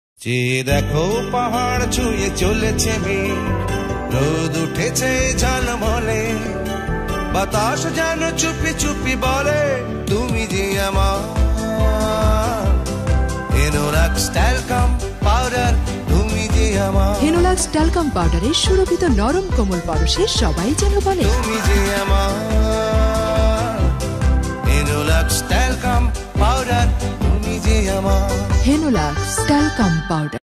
পুরনো টিভি এড শাকিব খান,পূর্ণিমা। হেনোলাক্স ট্যালকম পাউডার শাকিব পূর্ণিমা।
শাকিব খানের প্রথম বিজ্ঞাপন, এর আগে পরে শাকিব আর বিজ্ঞাপন করেন নি, পরবর্তিতে পাওয়ারের বিজ্ঞাপন করে৷